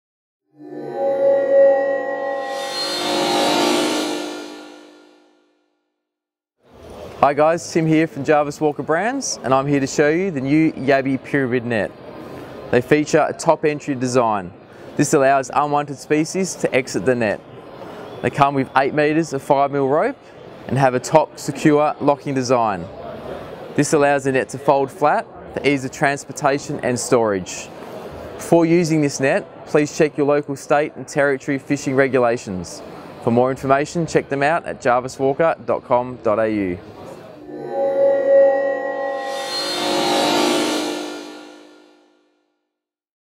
Yabby Pyramid Nets - Catch yabbies and avoid by-catch - AFTA 2019
Jarvis Walker Yabby Pyramid Nets offer a highly effective and environmentally friendly way to catch freshwater yabbies. The clever pyramid net design allows yabbies to drop through the top opening while the angled sides of the pyramid prevent their escape.

The large open-top also allows non-target species to enter and exit freely, minimising the potential for bycatch while still providing an efficient way to net yabbies. The net opening is also reinforced with a fixed wire frame to keep it in perfect alignment and to prevent the net from stretching out of shape.

The 600 x 600mm base and the triangular side frames slot together into a rigid form with the help of a secure-lock mechanism. The triangular articulation of the locking-slot also resists slipping and it acts as a perfect tie point for the included lift rope.

The Yabby Pyramid Nets are constructed with durable, high-quality mesh in a natural grey/green colour tone. The nets also feature a collapsible design for easy transport and storage.

Jarvis Walker Yabby Pyramid Nets offer a productive way to catch freshwater yabbies for use as bait or for a scrumptious feed. Simply tie in your preferred bait and watch the yabbies clamber in!